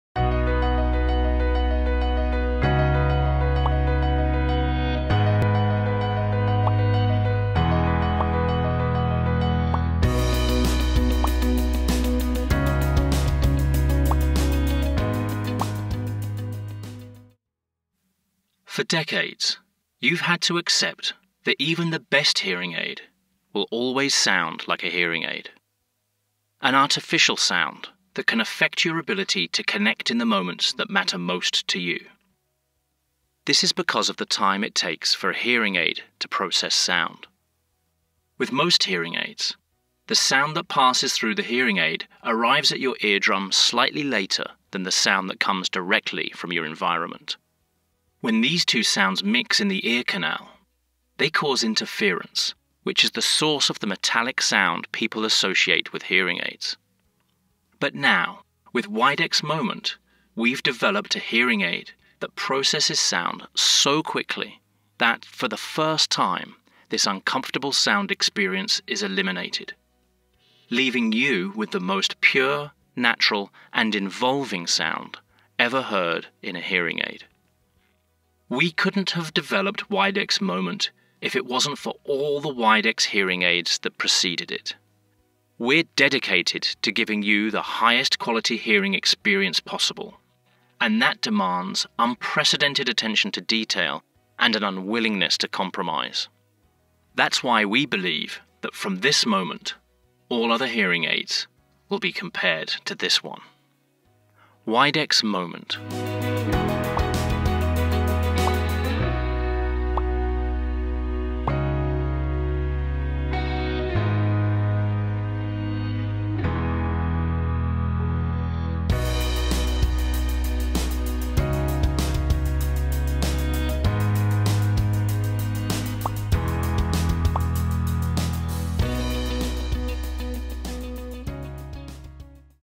Hoe klinkt een hoortoestel? Hoor zelf het verschil: Widex Moment-Natuurlijk & Onnatuurlijk
Vaak worden hoortoestellen als onnatuurlijk ervaren. Gelukkig bestaat er sinds kort een hoortoestel die WEL natuurlijk geluid geeft: De Widex Moment.

Gebruikers vinden het geluid van een hoortoestel vaak blikkerig. Hoe kan dat? En vooral: wat bestaat er al om dat op te lossen? En hoe klinkt dat dan?

Als er geluid via de microfoontjes het hoortoestel binnen komt, dan wordt het door de microcomputer  voor jou precies op maat versterkt. En vervolgens het oor ingestuurd. Deze bewerking in het gehoorapparaat kost enkele milliseconden. Je kunt je voorstellen dat het geluid wat langs de luidspreker rechtstreeks in het oor komt, eerder bij het trommelvlies is dan het hoorapparaat geluid. Er is dus een verschil tussen het geluid wat via het hoorapparaat komt en wat er rechtstreeks in het oor komt. Hierdoor ontstaat er een soort vertraging ofwel “faseverschil”. Dit verschijnsel maakt nou net dat het geluid onnatuurlijk klinkt.

Gelukkig hebben de ontwikkelaars van de diverse fabrikanten zich over dit probleem gebogen.

Inmiddels is er een hoortoestel, de MOMENT van Widex, die een nieuwe chip heeft die zo ontzettend snel is dat er nauwelijks meer een vertraging is tussen het hoortoestelgeluid en het rechtstreekse geluid. Hierdoor is men nu in staat om het nu echt natuurlijk te laten klinken. De ervaring is echt ongelofelijk.

Hier kan je het verschil horen tussen een normaal hoortoestel en het enige hoortoestel dat écht natuurlijk klinkt. Ontdek zelf het verschil!